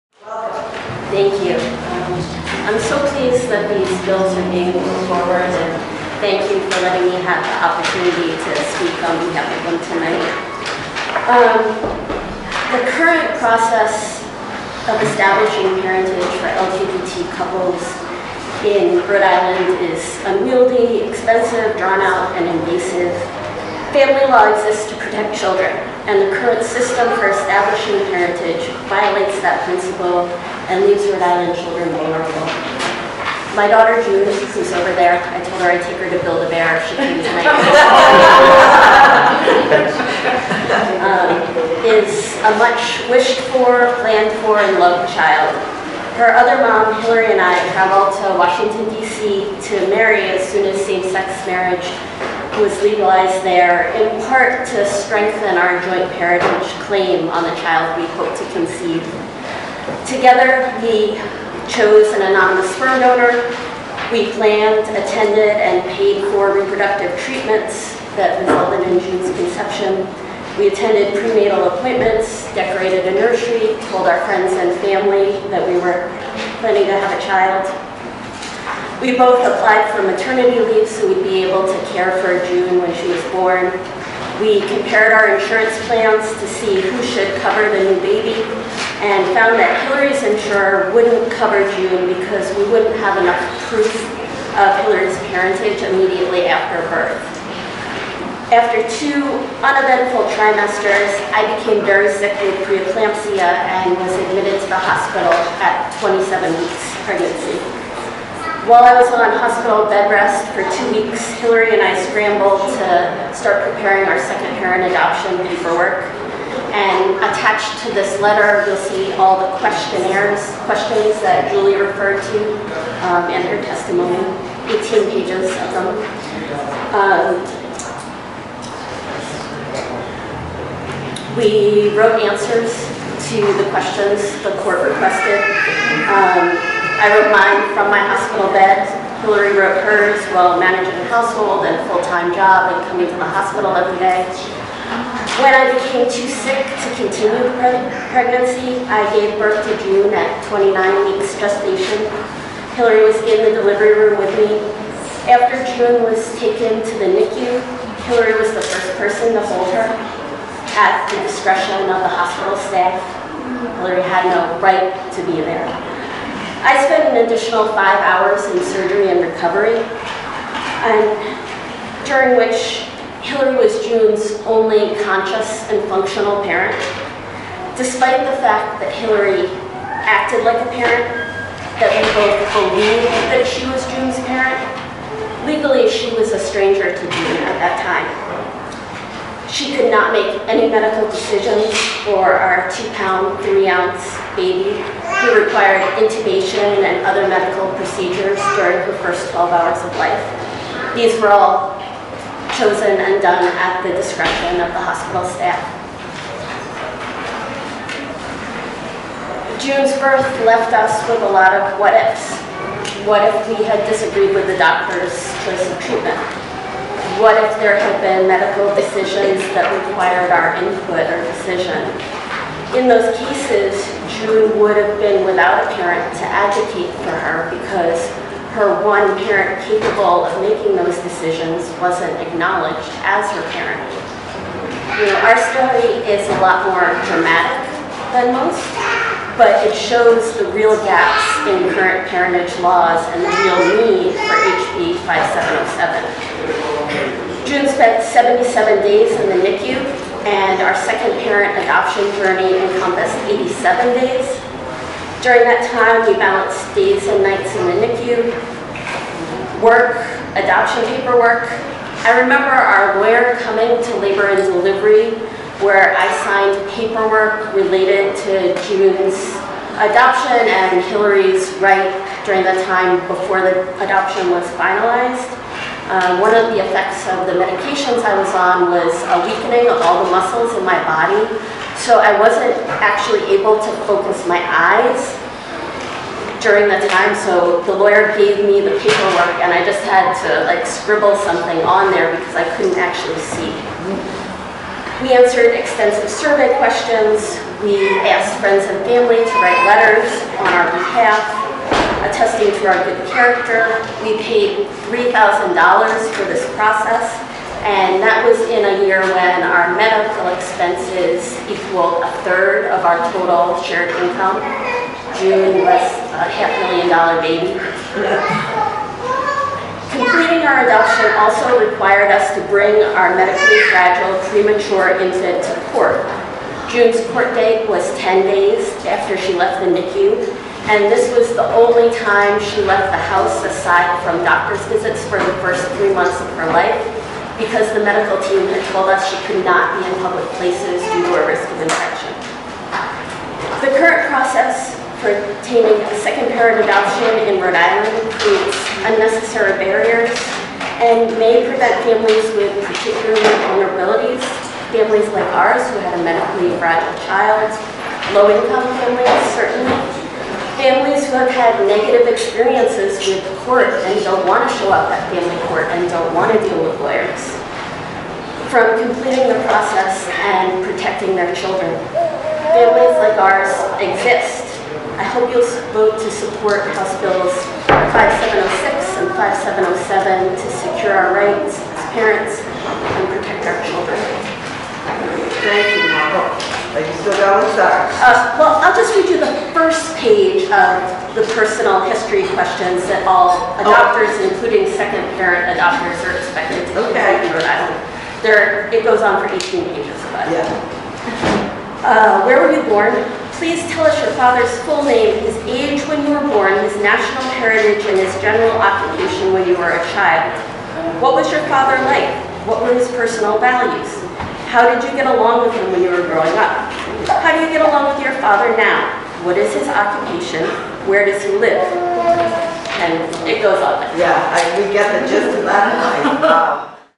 LGBT Parenting Battle: The Fight for Equal Parental Rights
A video from March 20, 2019, highlights the challenges faced by LGBT couples in establishing parentage under Rhode Island’s family law system. The footage features a same-sex couple, Laurie and Hillary, who share their personal experience navigating the legal complexities of second-parent adoption. The couple traveled to Washington, D.C., to marry when same-sex marriage was legalized there, aiming to strengthen their parental rights. They conceived their child, June, using an anonymous sperm donor and underwent reproductive treatments together. Despite both parents being actively involved in the pregnancy and birth, Hillary faced legal barriers in being recognized as June’s parent. After June was born prematurely at 29 weeks and spent 77 days in the NICU, the couple had to undergo an 87-day second-parent adoption process, which included significant financial and emotional burdens. The video underscores the gaps in current parentage laws, particularly for LGBT families, low-income households, and those with medically fragile children.